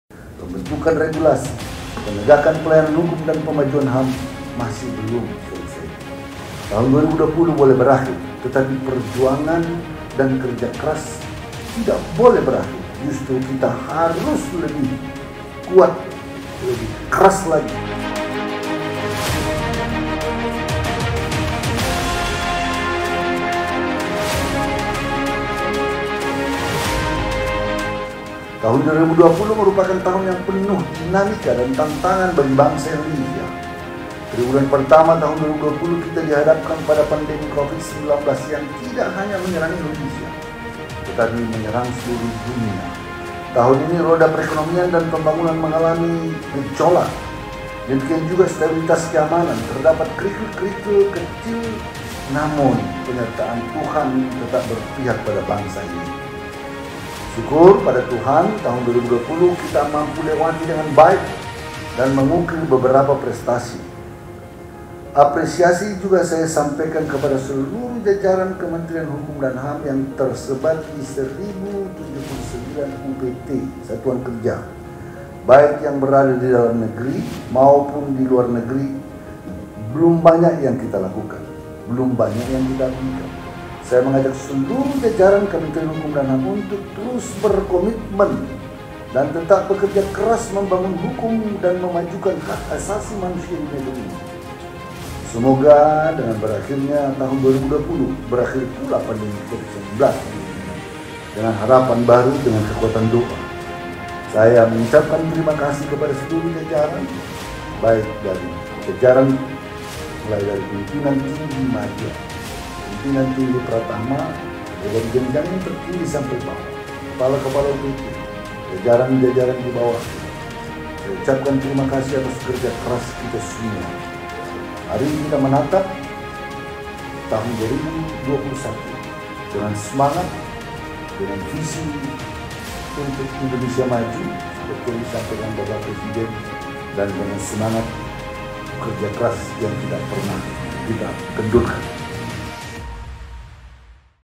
Menatap 2021 dengan Visi Indonesia Maju
Kerja keras seluruh jajaran Kemenkumham pada 2020 tidak boleh kendur dan harus ditingkatkan pada 2021. Tingkatkan layanan publik dan bersama wujudkan Kemenkumham yang lebih Pasti.